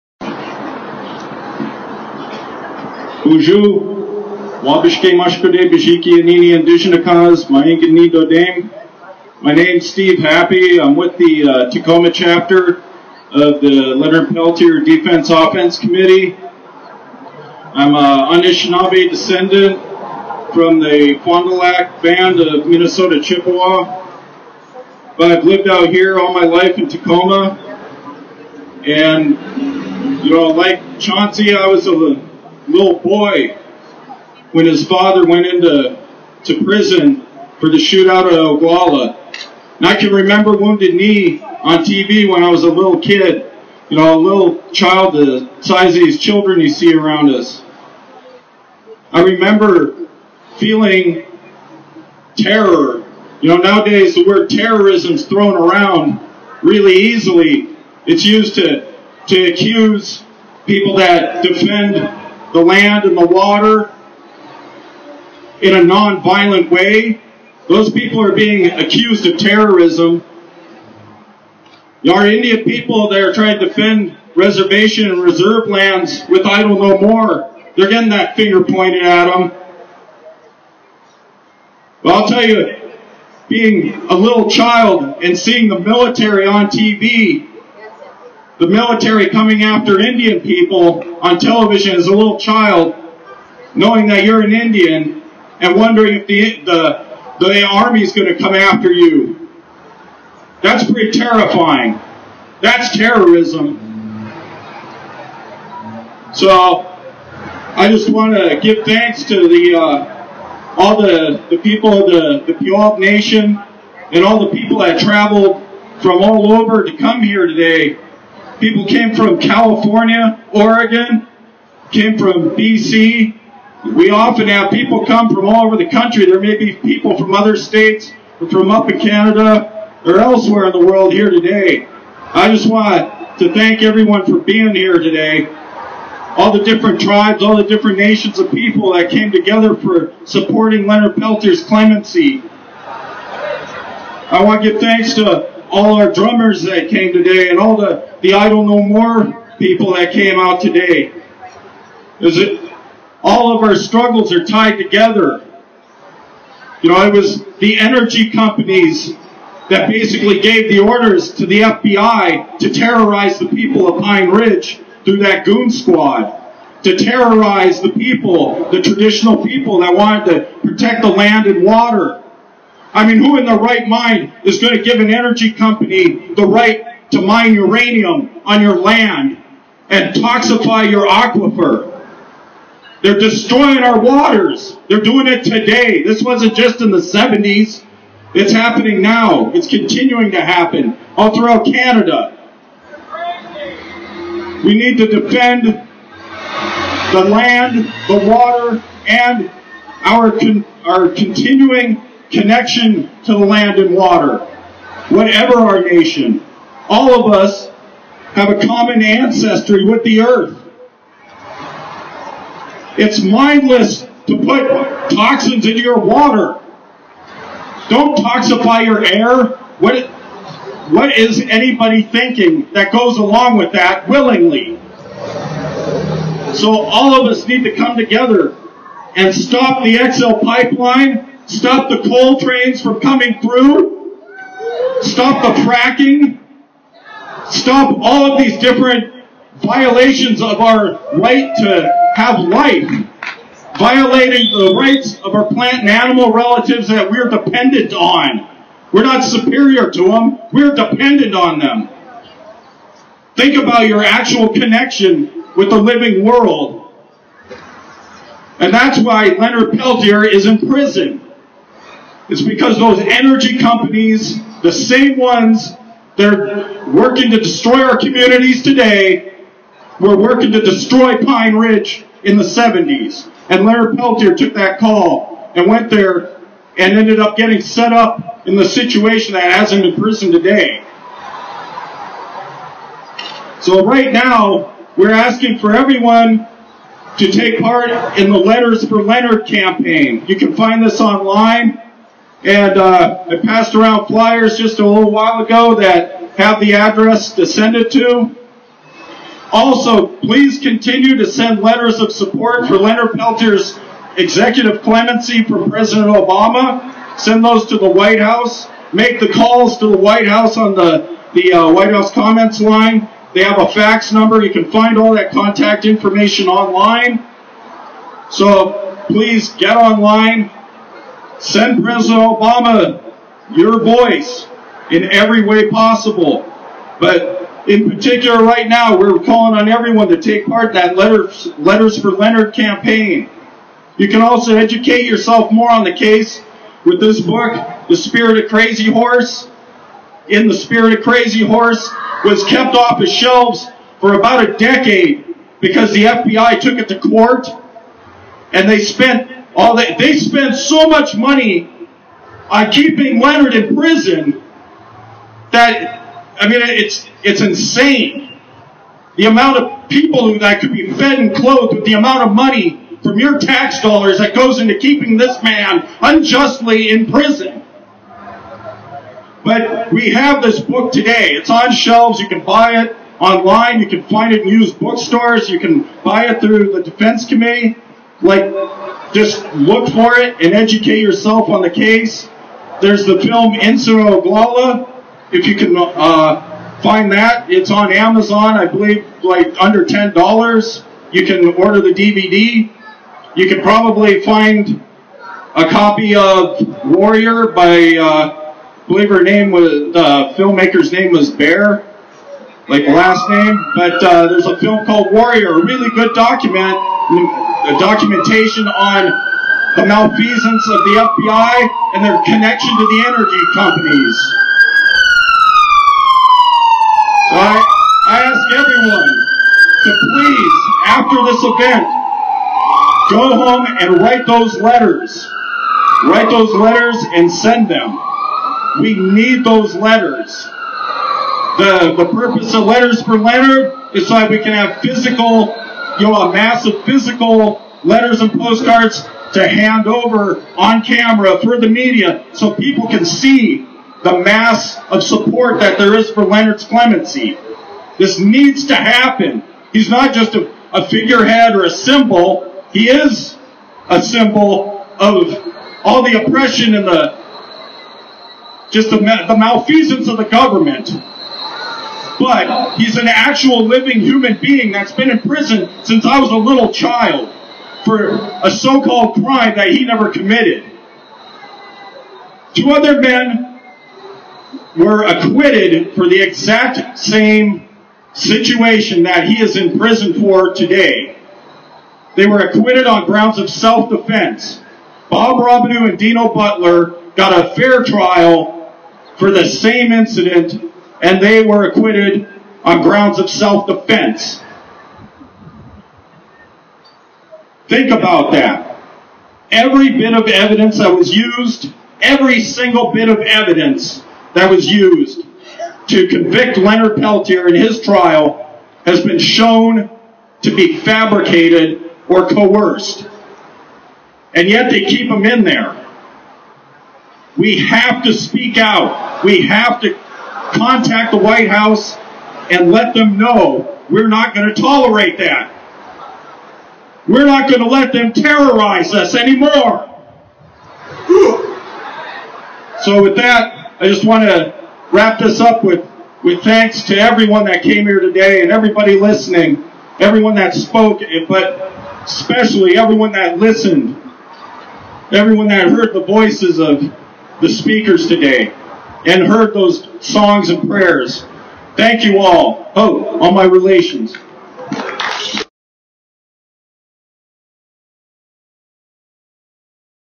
Steve Hapy
Long time Leonard Peltier and Native struggles activist, Steve spoke at the rally in Tacoma for Leonard Peltier and Mother Earth.